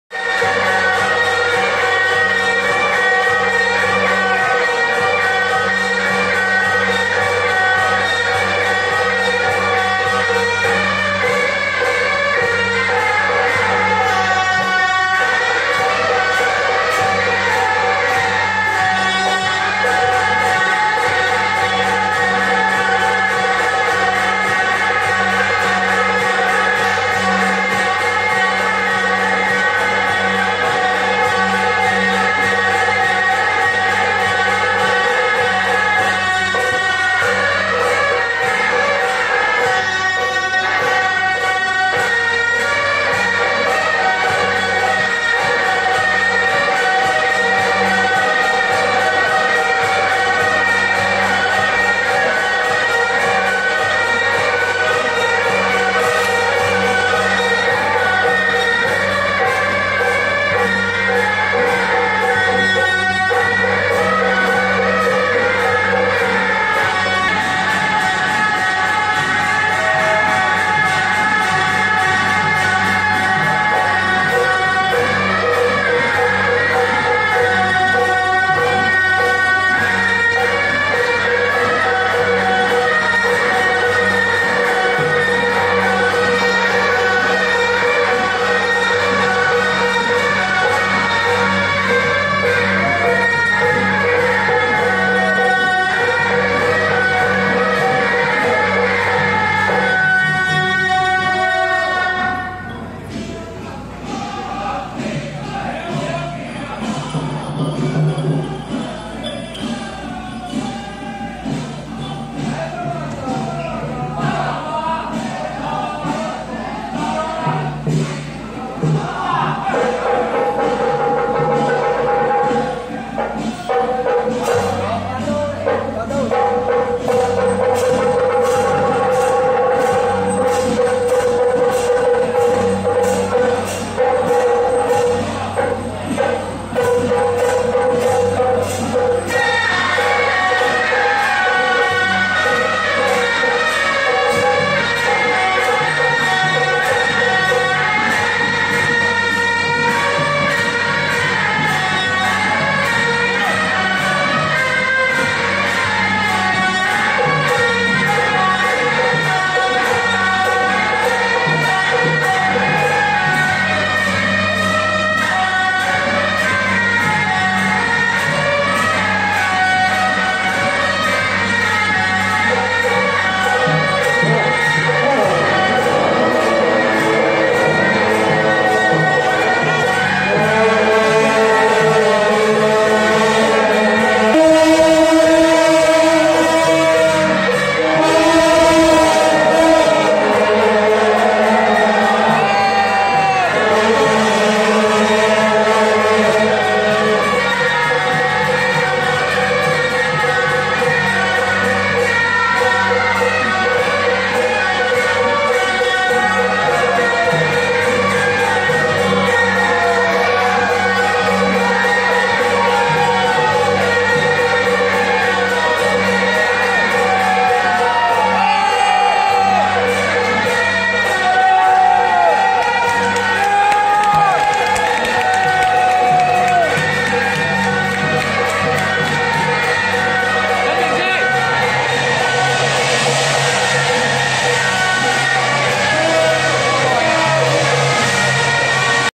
107.6/3台北下太子閤聖會中壇元帥南巡謁祖進香回駕遶境【台北下太子閤聖會-主帥轎~中壇元帥 聖駕(臺北双聯轎班會)】
台北下太子閤聖會-主帥轎~中壇元帥 聖駕(臺北双聯轎班會)